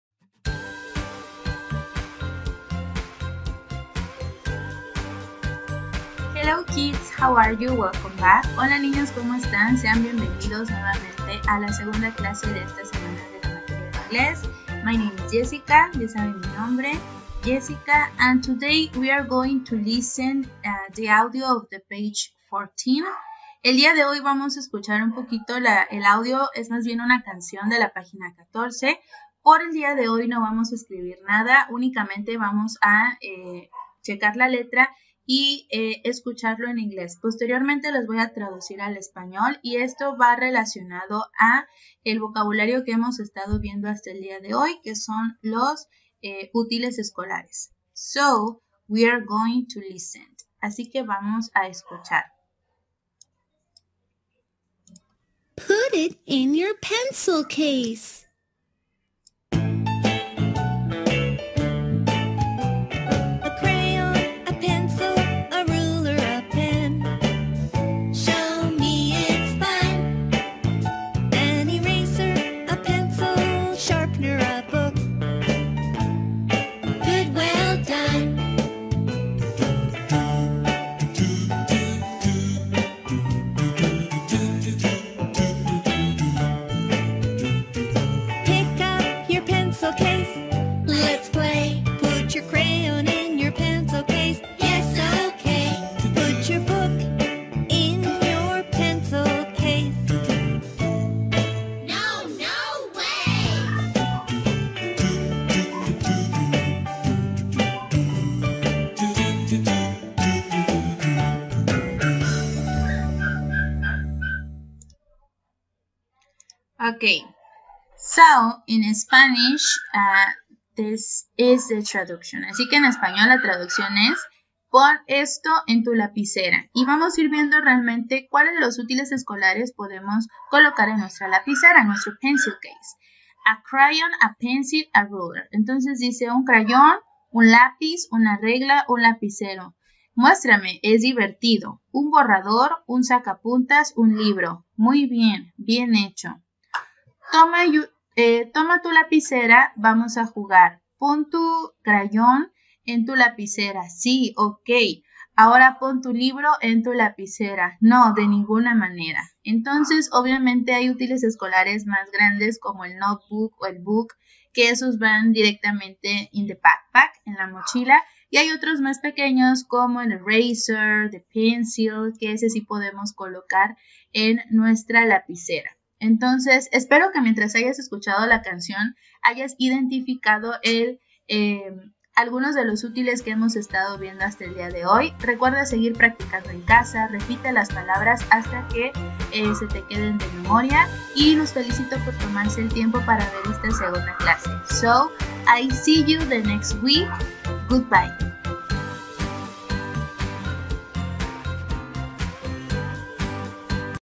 1er grado. Segunda clase del 3 al 6 de Noviembre.
Hello kids! El día de hoy escucharemos una canción sobre los útiles escolares, identifica los que ya conoces y recuerda practicar la pronunciación en casa.